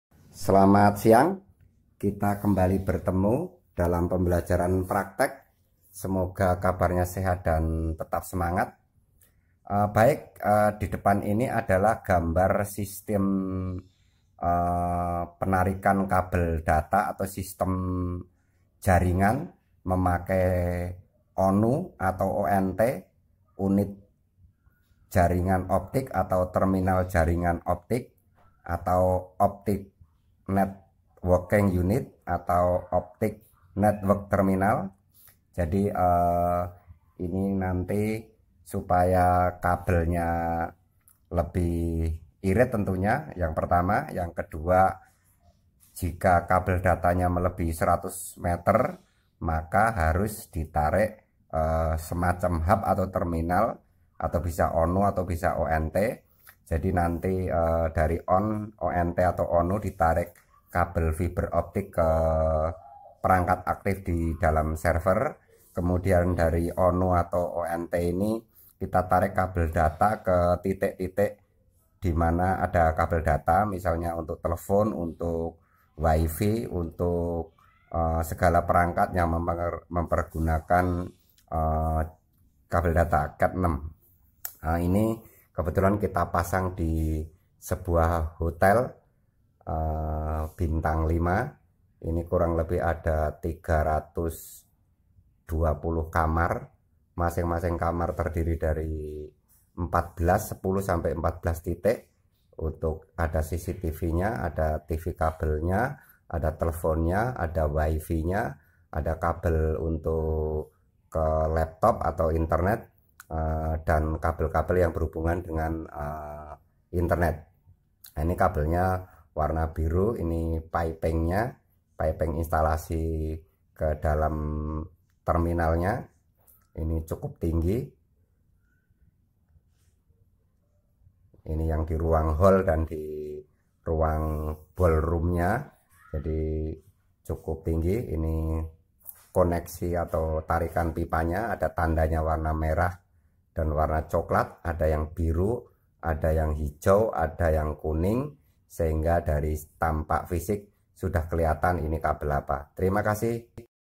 Memasang jaringan ONU/ONT di hotel, pembelajaran praktek.
Jaringan optik pasif terdiri dari terminal jalur optik (OLT) di kantor pusat penyedia layanan (hub) dan sejumlah unit jaringan optik (ONU) atau terminal jaringan optik (ONT), di dekat pengguna akhir. PON mengurangi jumlah serat dan peralatan kantor pusat yang dibutuhkan dibandingkan dengan arsitektur titik-ke-titik . Jaringan optik pasif adalah bentuk jaringan akses serat optik. sumber wiki